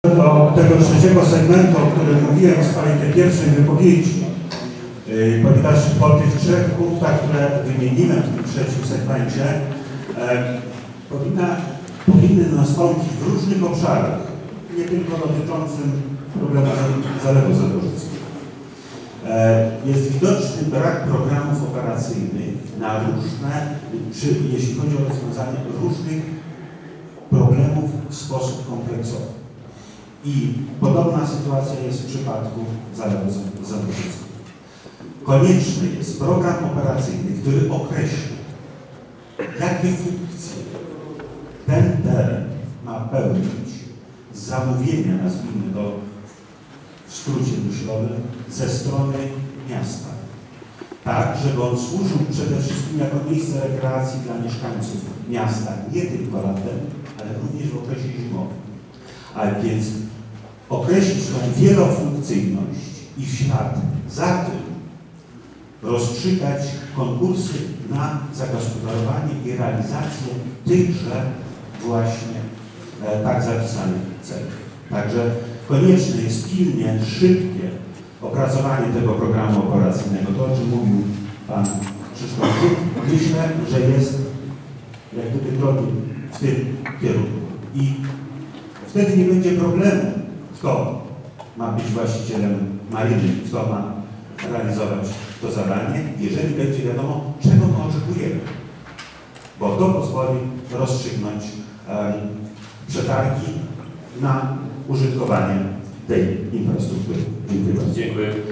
Sprawka: nie wiadomo jaka jest funkcja Zalewu
Debata kandydatów na prezydenta Lublina, 27 września